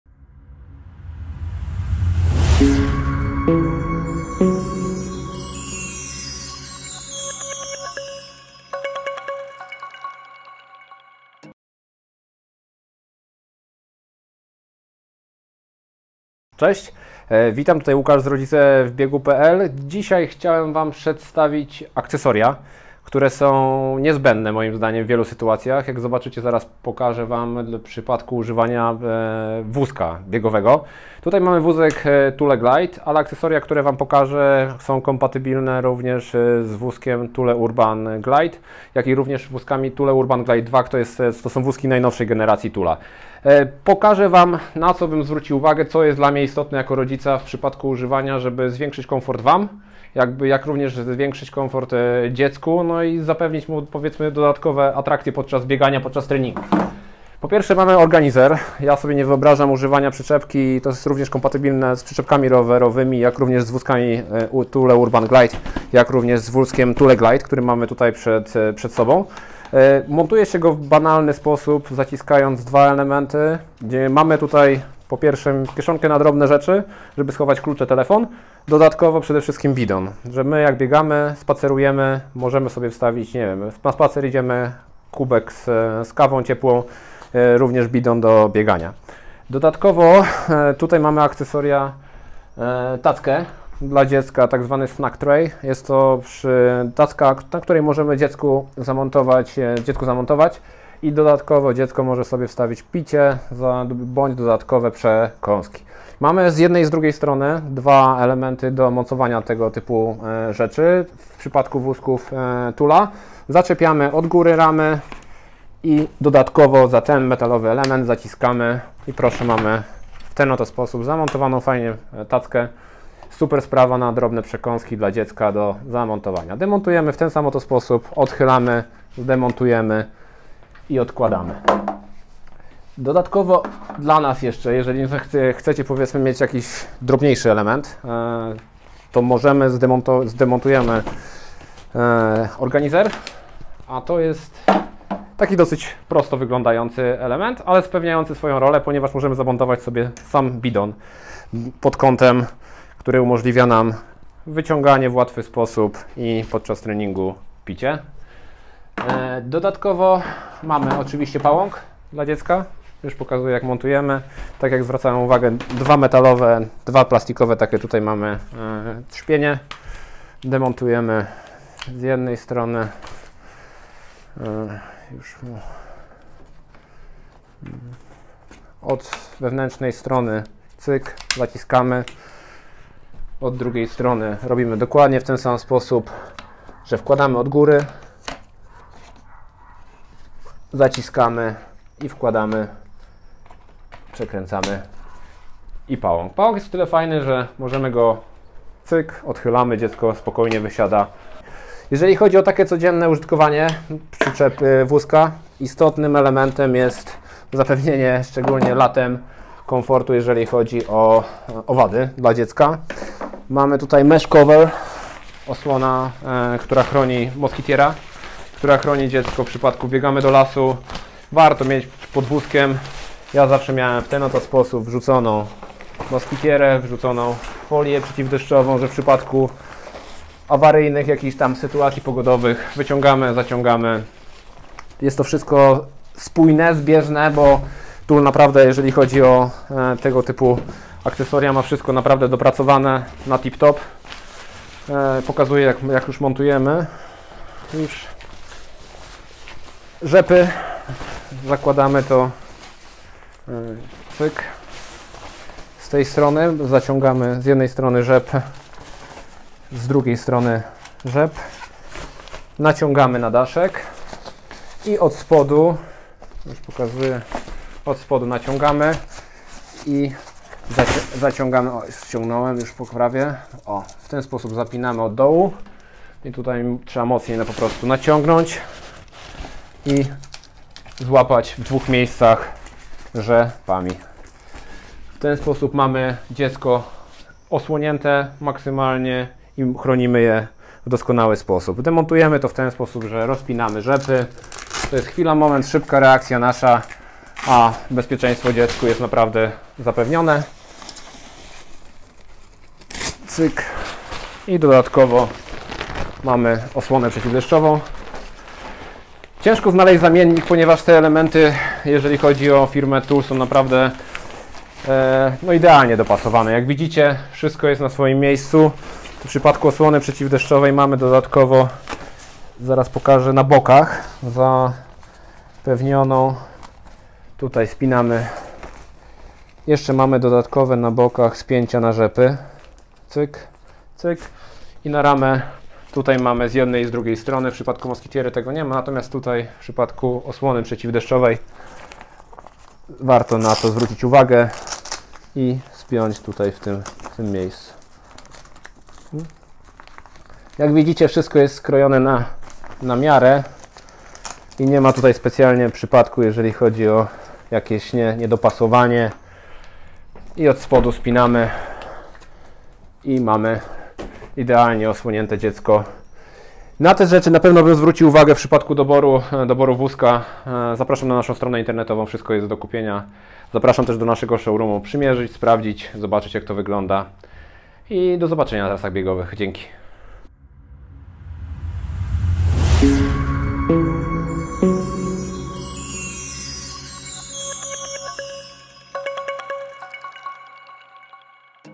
Akcesoria THULE: konsola x uchwyt x pałąk x Snack Tray x moskitiera x osłona przeciwdeszczowa
W dzisiejszym odcinku bierzemy na warsztat:
- konsola
- uchwyt na kubek
- pałąk
- Snack Tray
- moskitiera
- osłona przeciwdeszczowa